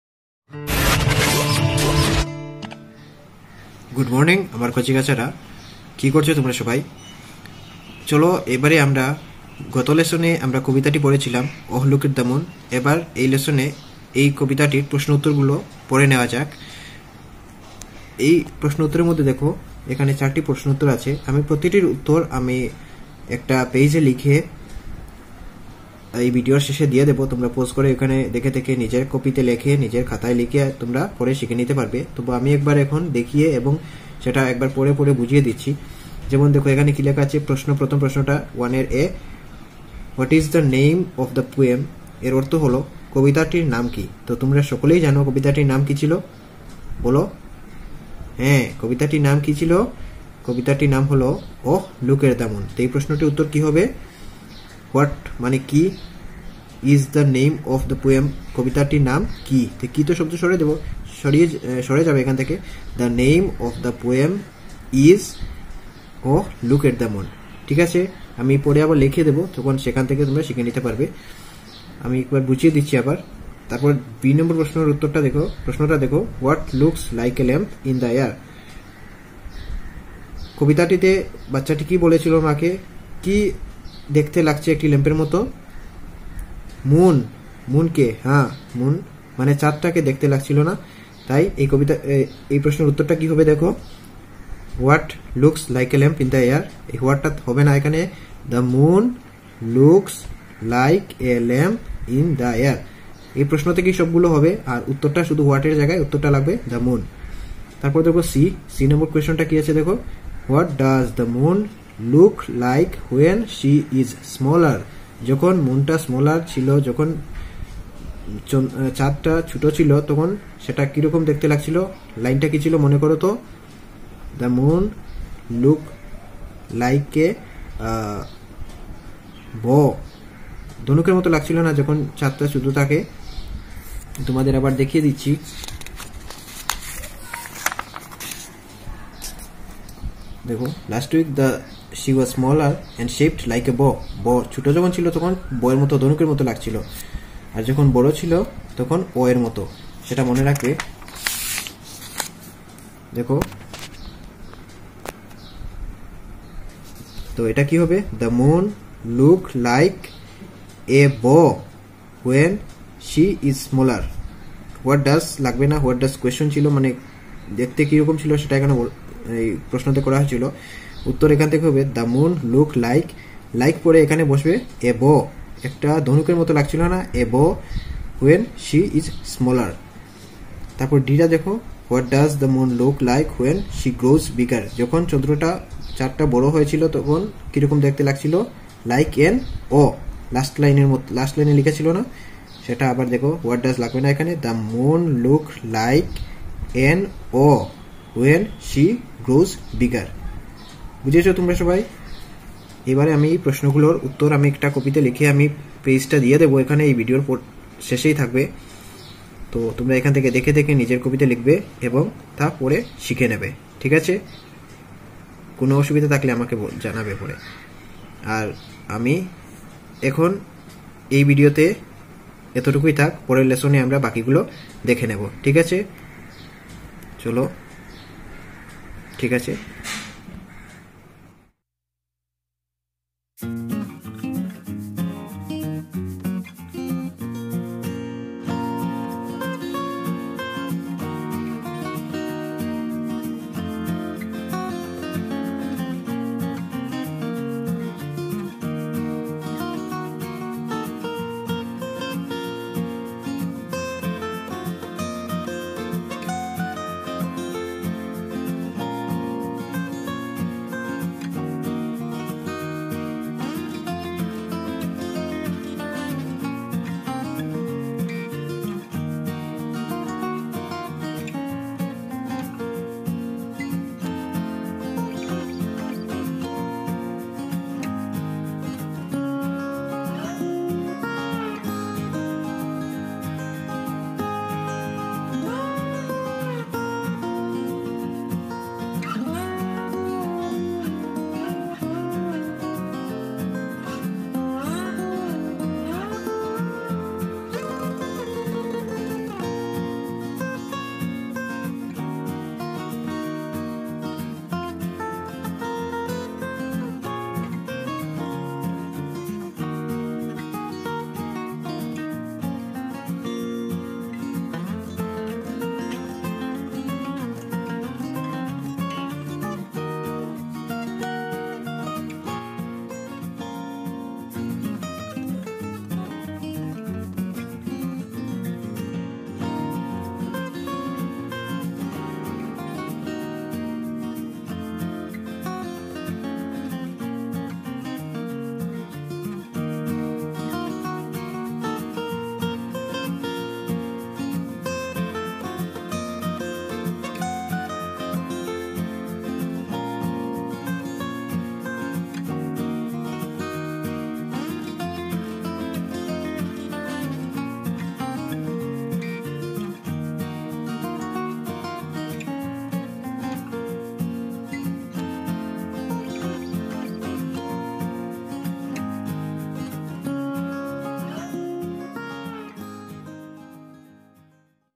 Sub- English | Class-4 | Lesson-1 (V-2) | Page No-10 | Video Classes for LP Students during Lockdown
Academic Support Videos for Lower Primary  School Students (Govt. ) During Lock Down Period..

লকডাউনের সময়কালে নিম্ন-প্রাথমিক বিদ্যালয়ের শিক্ষার্থীদের শৈক্ষিক সহায়তার জন্য কিছু বিষয়ের উপর তৈরি করা কিছু ভিডিও ক্লাস..